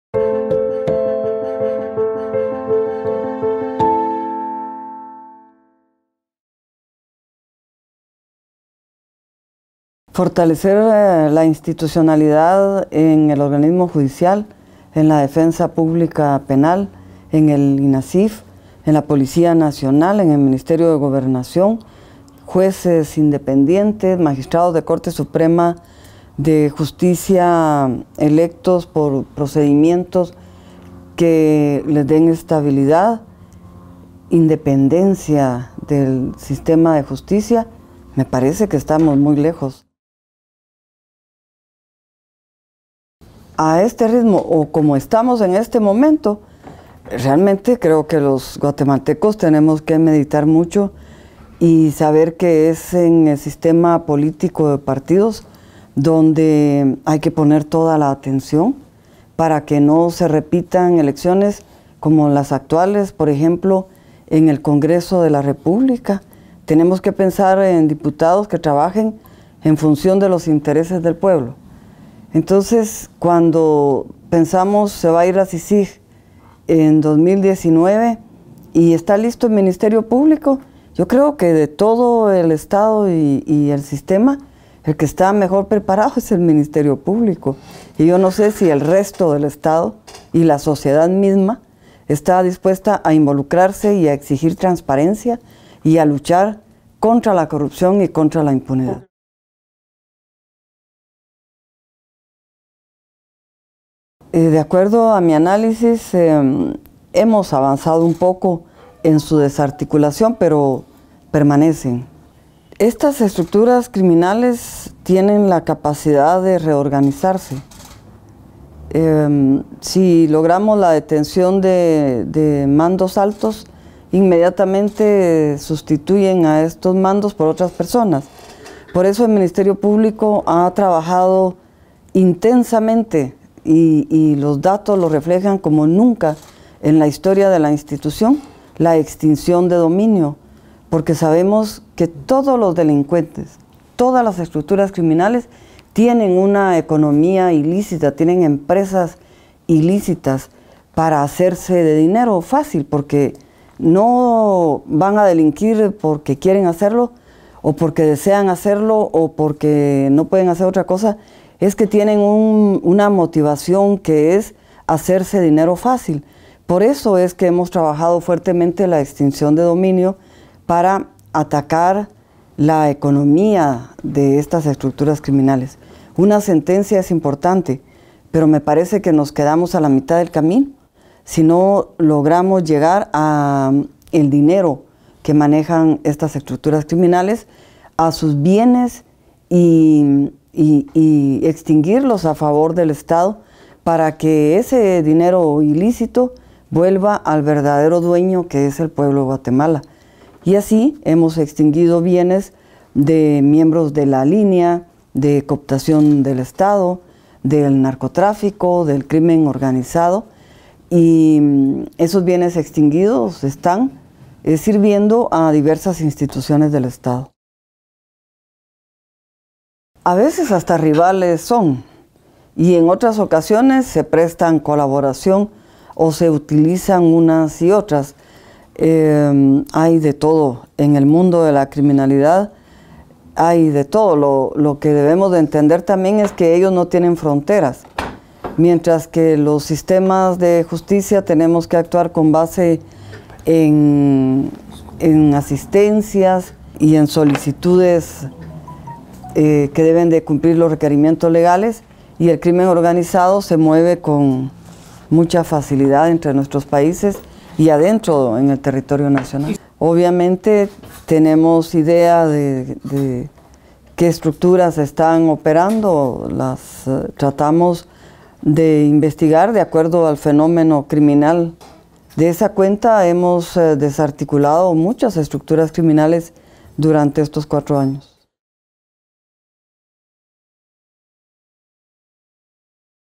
Entrevista a Thelma Aldana | El futuro del MP y la Cicig
Parte 2. La titular del MP habla sobre el tiempo que considera que debe estar la Cicig, además de los temas pendientes de fortalecer en el sector justicia. (Video Prensa Libre: Álvaro González)